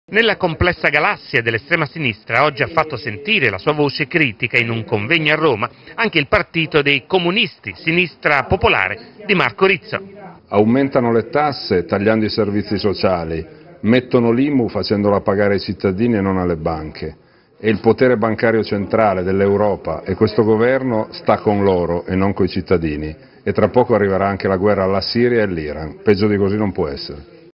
Marco Rizzo: deciso no alle politiche del governo Monti
Marco Rizzo, segretario di CSP Partito Comunista fa una profonda critica al Governo di Mario Monti e denuncia l'arcana imperii dell'Europa e delle banche. Deciso no all'IMU e alla guerra ormai prossima in Iran e Siria.